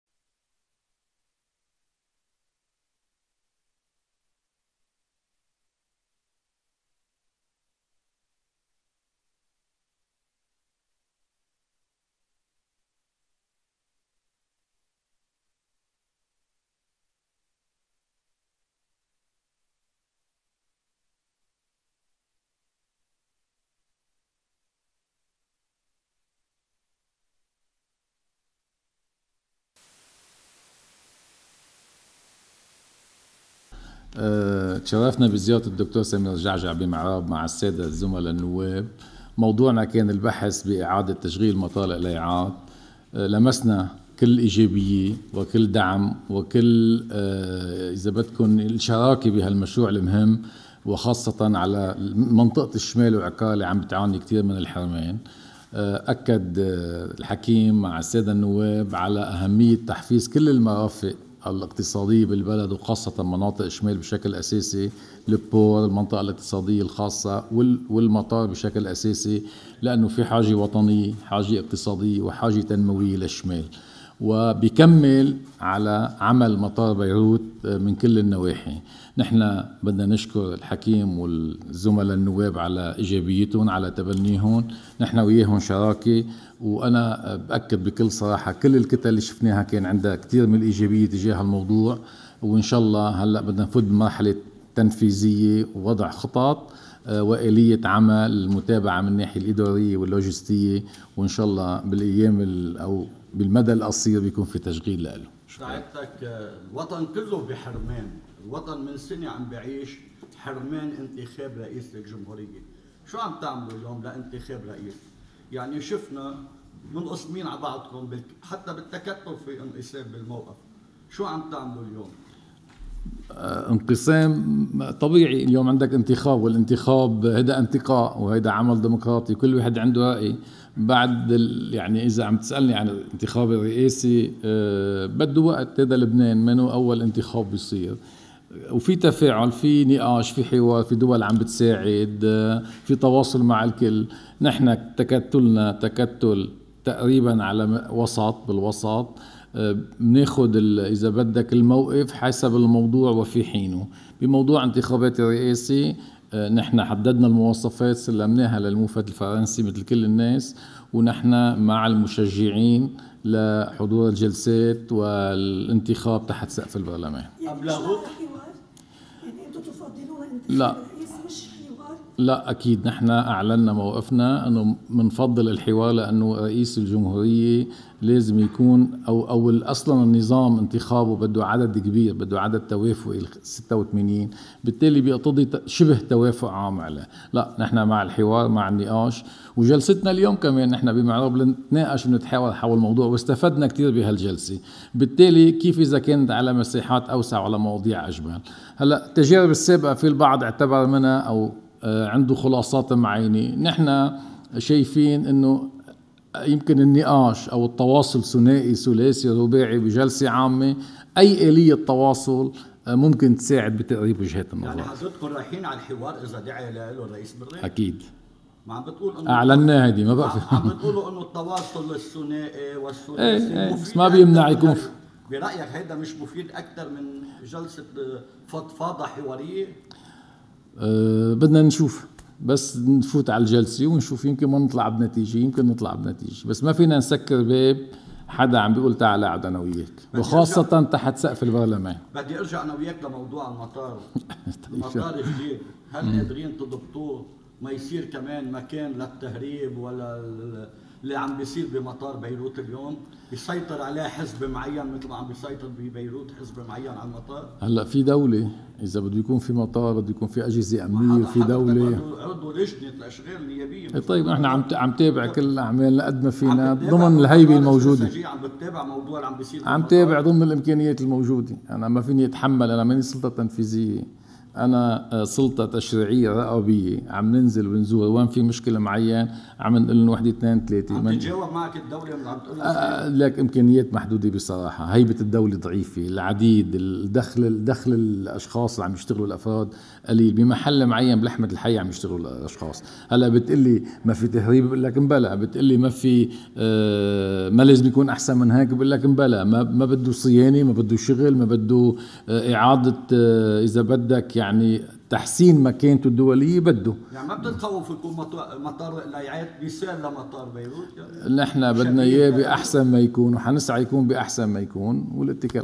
عطيّة: جميع الكتل التي التقيناها كان لديها الكثير من الإيجابيّة إزاء إعادة تشغيل مطار القليعات
لتقى رئيس حزب "القوّات اللبنانيّة" سمير جعجع، في المقر العام للحزب في معراب، وفداً من تكتل "الإعتدال الوطني" ضم النواب: وليد البعريني، محمد سليمان، سجيع عطية والنائب السابق هادي حبيش، في حضور أعضاء تكتل "الجمهوريّة القويّة" النواب: غسان حاصباني، فادي كرم ، أنطوان حبشي وايلي خوري.
عقب اللقاء وضع عطيّة الزيارة في إطار البحث  " بإعادة تشغيل مطار القليعات وقد لمسنا من "الحكيم" كل إيجابيّة ودعم وشراكة في هذا المشروع المهم خصوصاً لمنطقة الشمال وعكار التي تعاني الكثير من الحرمان".
 أضاف" كما أكد  "الحكيم" مع السادة الزملاء النواب في تكتل "الجمهوريّة القويّة" على وجوب تحفيز كل المرافق الإقتصاديّة في البلاد ولا سيما في منطقة الشمال بشكل أساسي، أكان المرفأ أو المنطقة الإقتصاديّة الخاصة والمطار بشكل أساسي في ظل الحاجة الوطنيّة والإقتصاديّة  والتنمويّة لهذه المنطقة ، باعتبار أن مطار القليعات يأتي مكملاً لعمل مطار بيروت في شتى النواحي".
عطيّة الذي شكر جعجع ونواب القوات على تبنيهم هذا المشروع، لفت إلى" أن جميع الكتل التي التقيناها كان لديها الكثير من الإيجابيّة إزاء هذا المشروع لأهميته ، انطلاقاً من هنا ، سندخل الآن في المرحلة التنفيذيّة ووضع الخطط وآلية عمل المتابعة من الناحيتين الإداريّة واللوجستية، وإن شاء الله سنتمكّن من تشغيل المطار في المدى القريب".